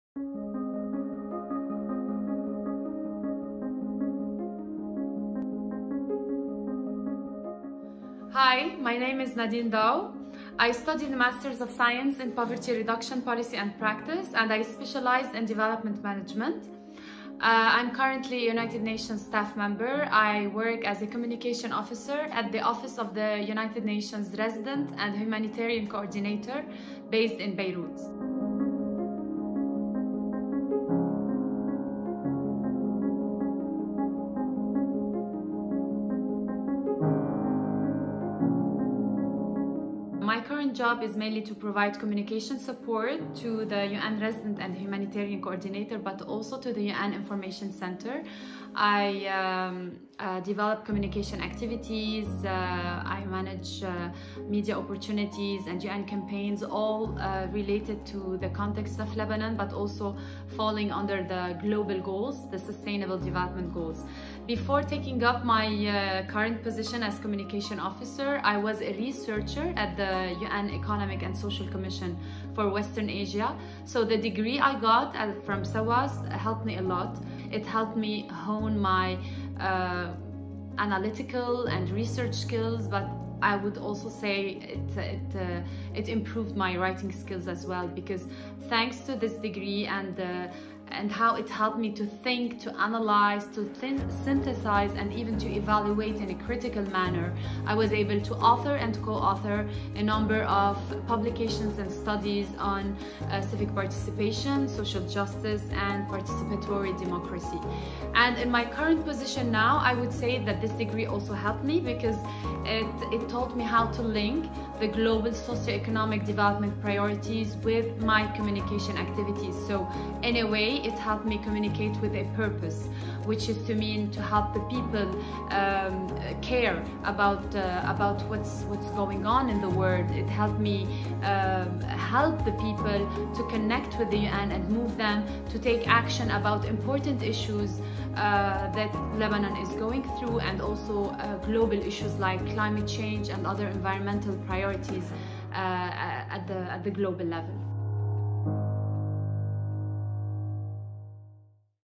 Nadine R. Abi Zeid Daou, Centre for Development, Environment and Policy Student Profile
We spoke with Nadine R. Abi Zeid Daou who shares her Centre for Development, Environment and Policy student experience.